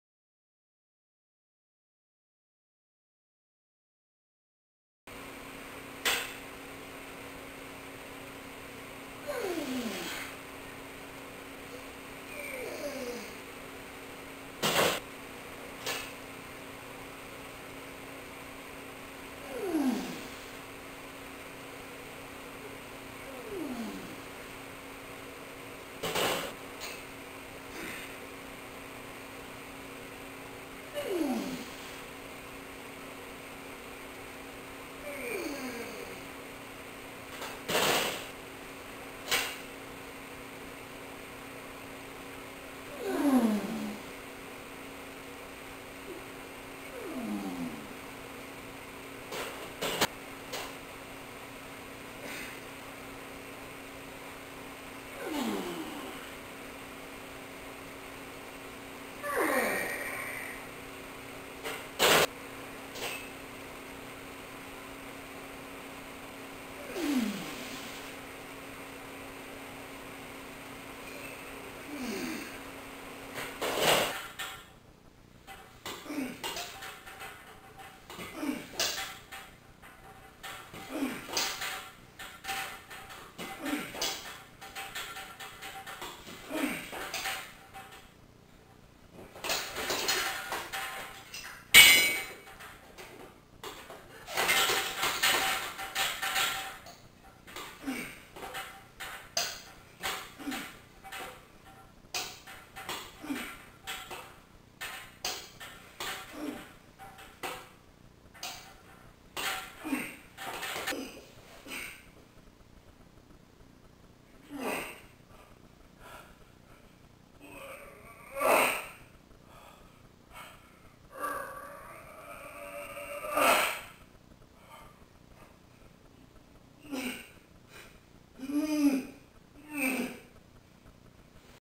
Rus SQ/BP w3d4 bench press 254 6 sets of 2
Bench press
WU 44 x 20,20 143 x 5, 187 x 3, 231 x 2
254 6 sets of 2

Parallel grip pulldowns
WU 75 x 10, 100 x 8
130 x 5, 150 x 5, 165 x 5, 185 x 5, 200 x 5, 215 x 5; drop to 165 x 5

The last set was a drop set. It took me three workouts to get my reps in on the last set.  Now that I have finally done it I am going to move to wide grip front pulldowns for a bit.

Seated dumbell press
WU 30 x 8, 40 x 5
55 x 5, 60 x 5, 70 x 5, 80 x 5, 85 x 4, 90 x 4

Second to last set was a bit rough.  The last couple weeks these have been tough.  I haven't made it to the fifth and six sets.  This was my first run with the 90s.  We will see how it goes next time.